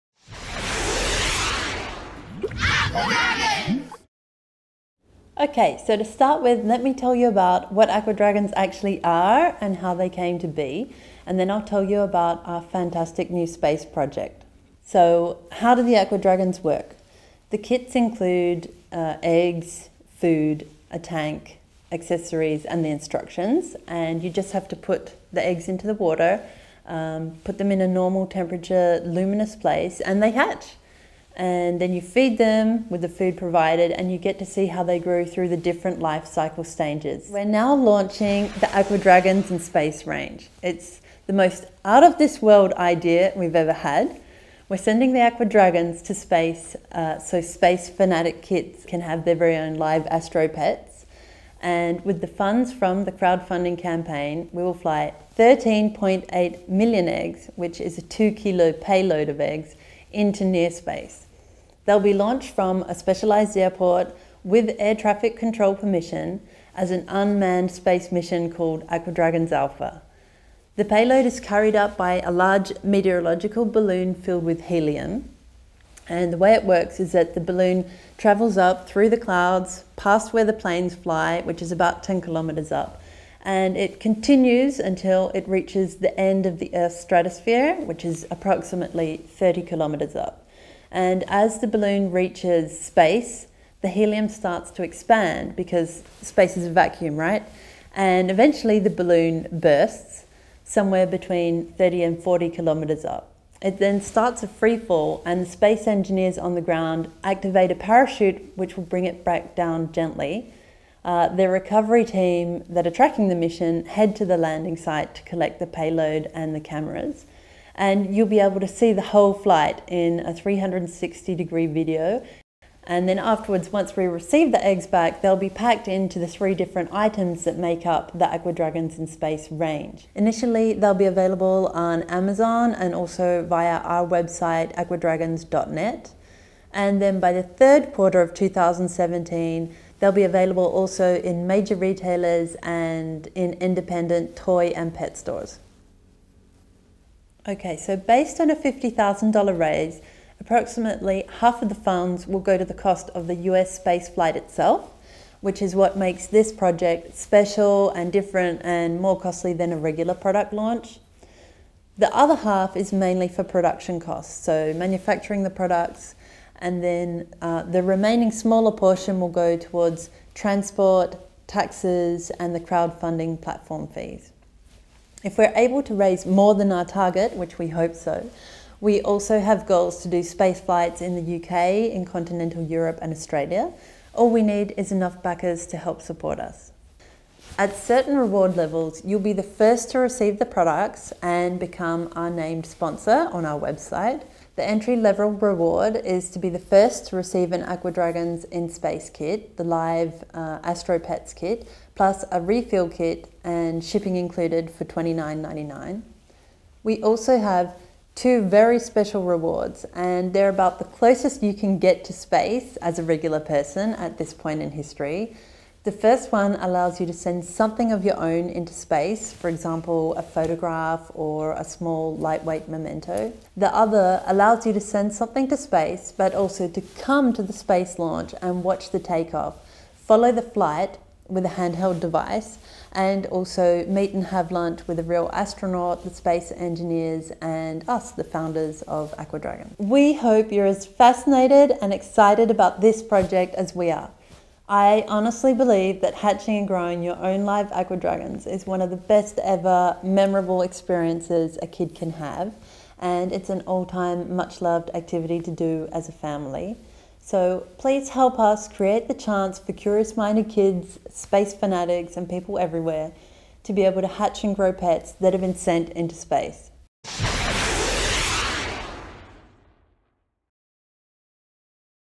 Aqua Dragons in Space Kickstarter project video
collaborate and help send the Aqua Dragons and maybe something of your own to Space, yes real Space!